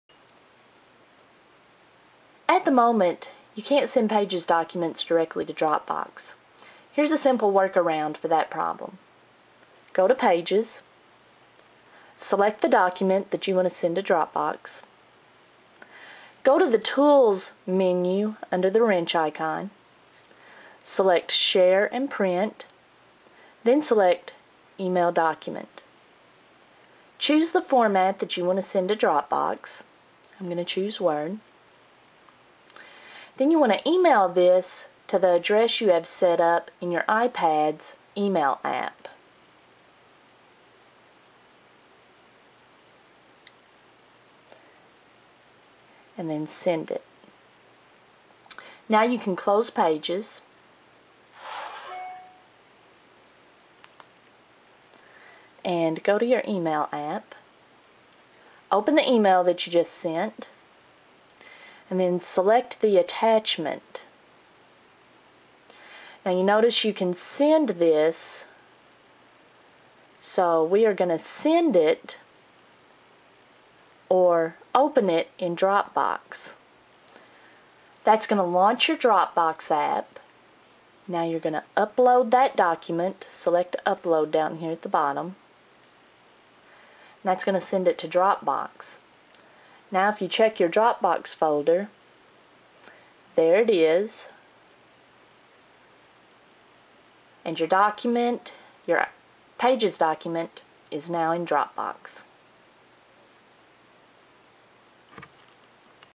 Send Pages documents from an iPad to Dropbox-Update!
There is now an easier way:
1-Go the wrench icon and click share and print
2-Choose open in another app
3-Select the file type (Pages, Word)
4-Select an app that can open that file type; if you have Dropbox then it will be an option.